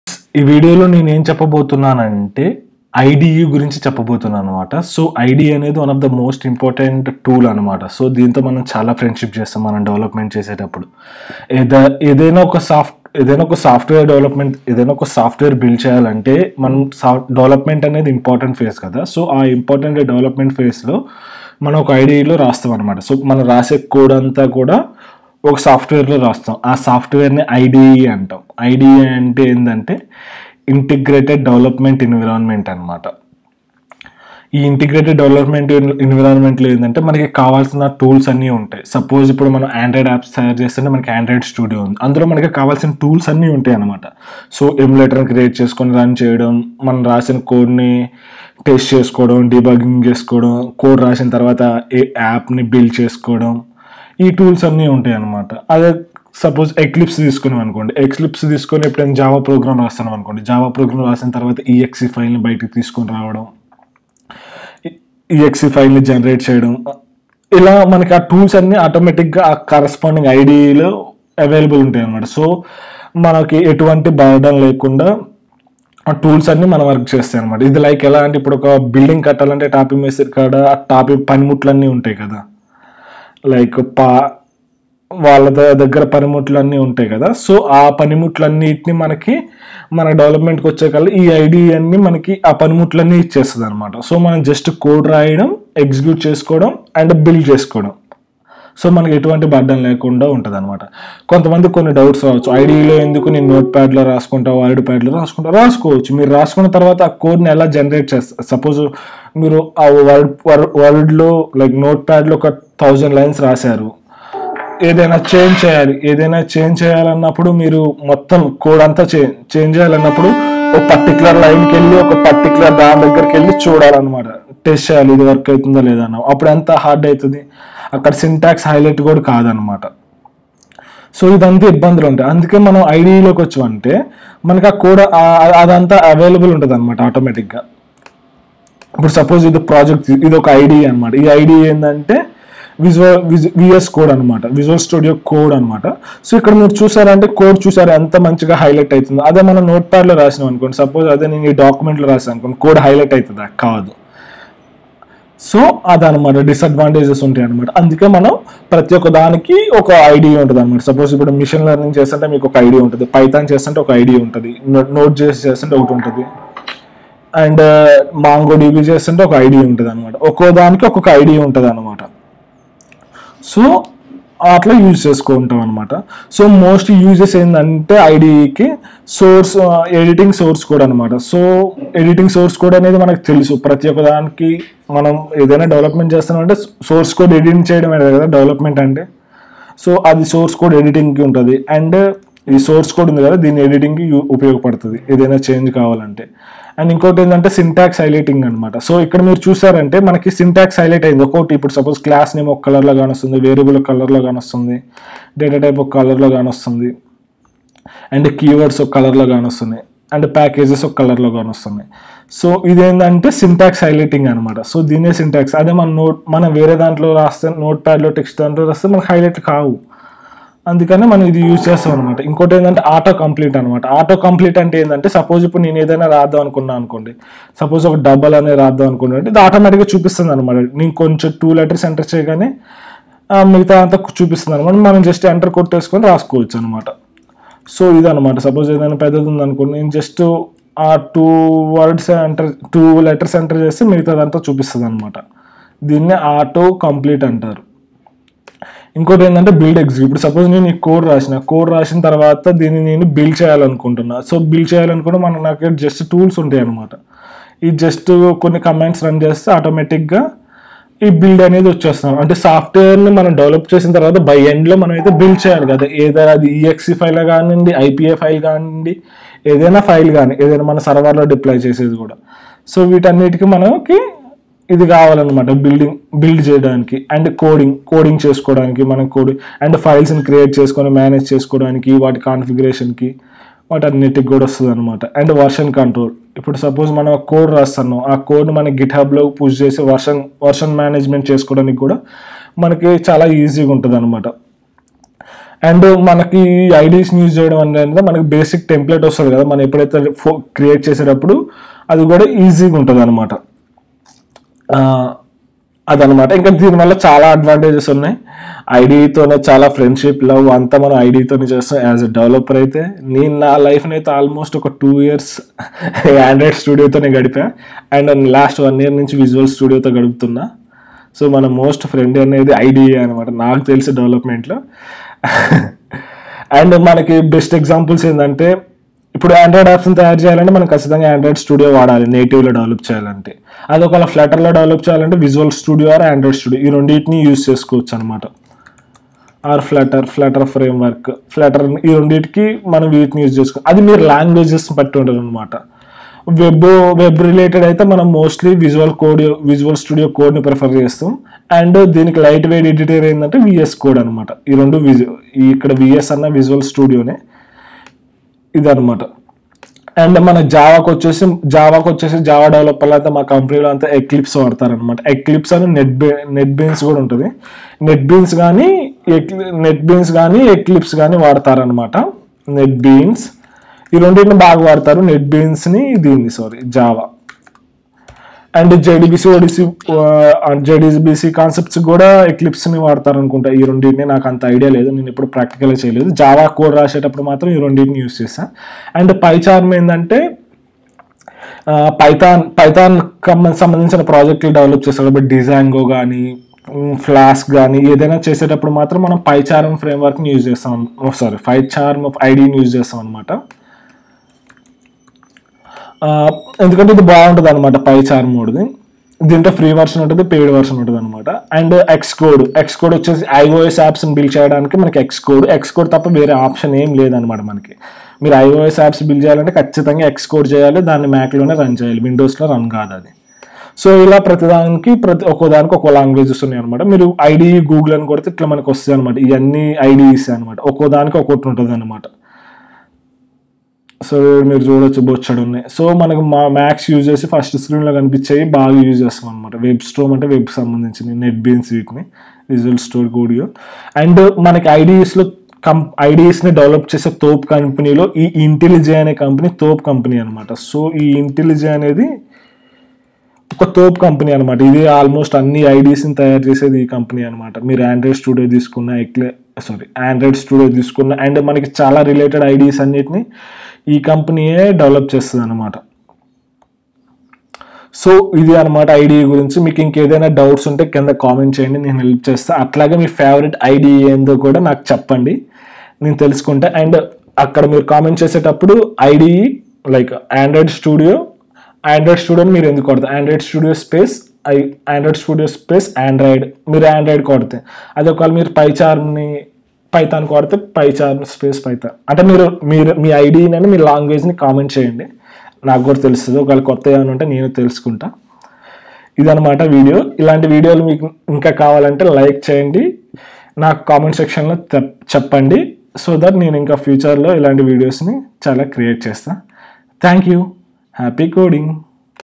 What is an IDE | Importance of IDE in Software Development with Examples in Telugu | Sai Gopi
What is IDE and which is should i use for particular langauge explained with live and real time examples in this video.

IDE - Integrated Development Environment

Uses of the IDE:
Editing Source Code
Syntax highlighting
AutoComplete
Building Executable
Coding
Version Control
So on …

Example:
Android Studio - Android Apps or flutter
VS Code - java,js,python,dart,flutter,so on.
Visual Studio Code -  java,js,python,dart,flutter,so on
Eclipse - Java,so on,
Netbeans - java
PyCharm - Python
XCode - iOS Apps,
So on